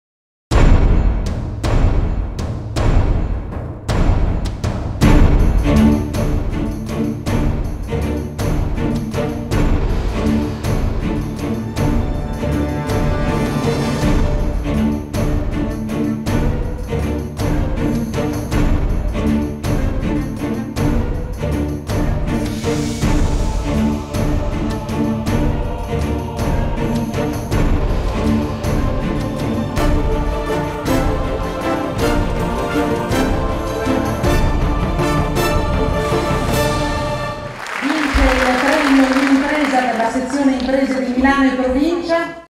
Premio YouImpresa 2015
16 marzo 2015 - Torna la VI edizione del Premio YouImpresa. Porta il tuo video aziendale alla Scala di Milano. Il prossimo vincitore, potresti essere tu!